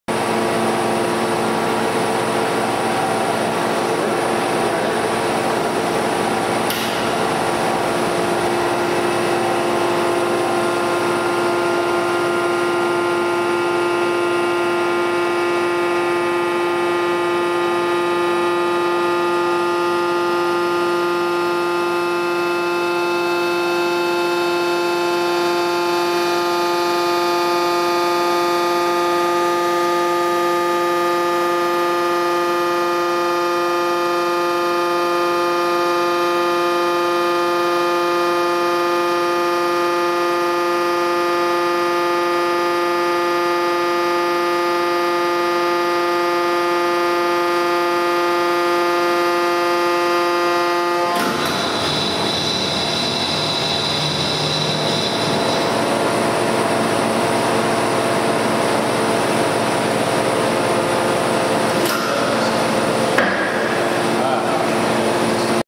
Test Deformation Buffer.mpg
Testing of deformation tube for metros and light railways